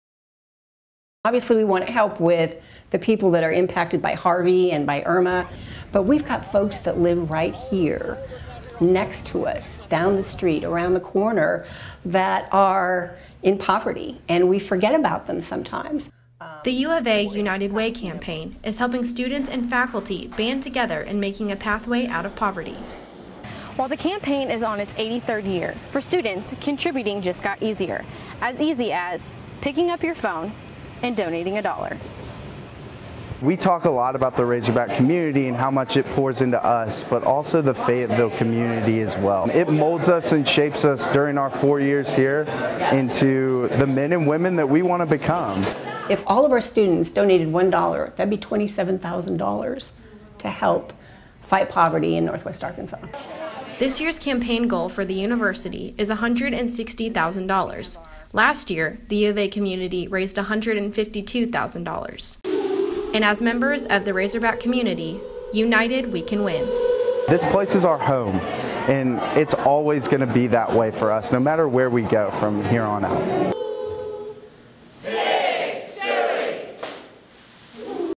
UNITED WAY NWA Campaign
UATV news story featuring United Way Campaign at the University of Arkansas. Helping the thousands of citizens in Northwest Arkansas living in poverty